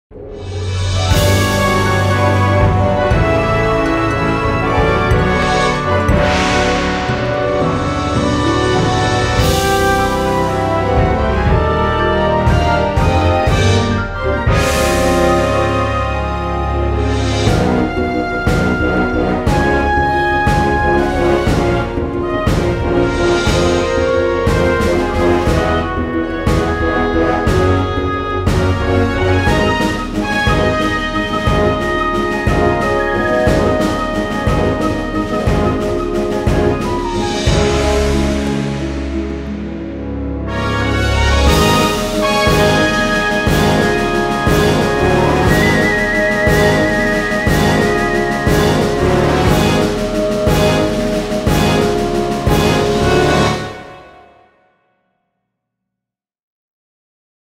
Epic Orchestral Cue
This is 1 of 3 cues I made for a film scoring competition.